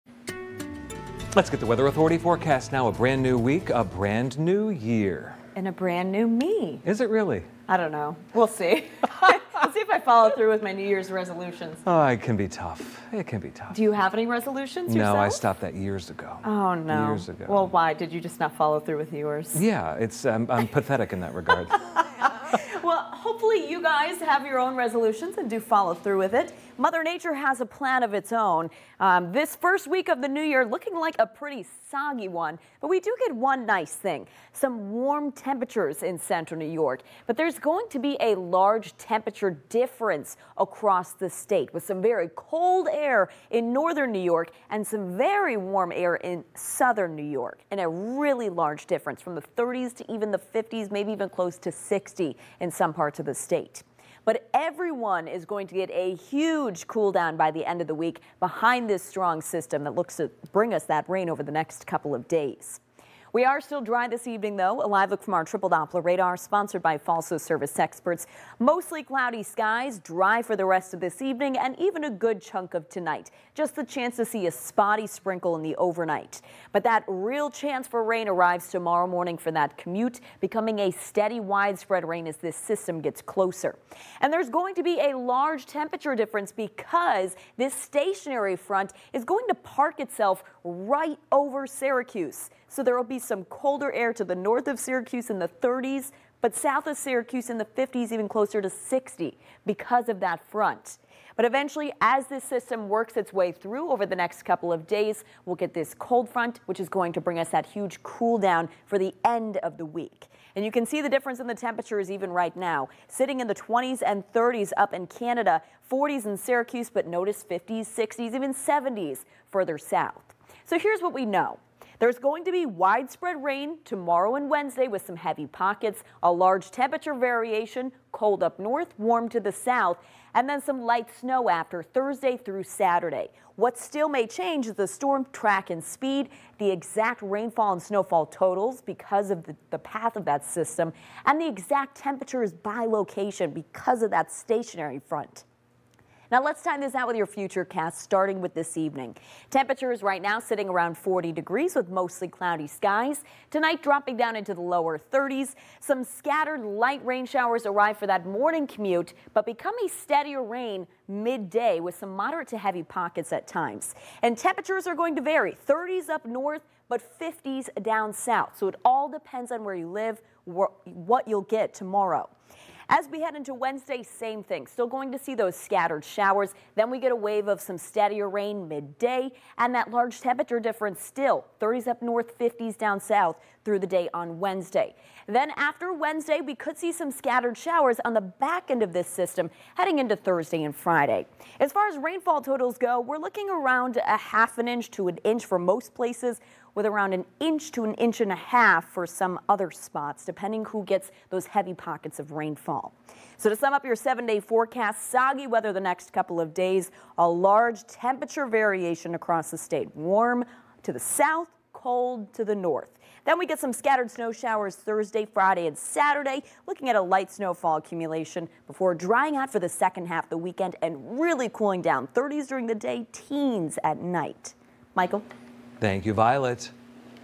Soggy system and a big temperature difference!
A strong system is on the way with some soggy weather for the next couple days. Plus, a stationary front will sit horizontally over Syracuse with a huge temperature difference across the state! Watch the video for details.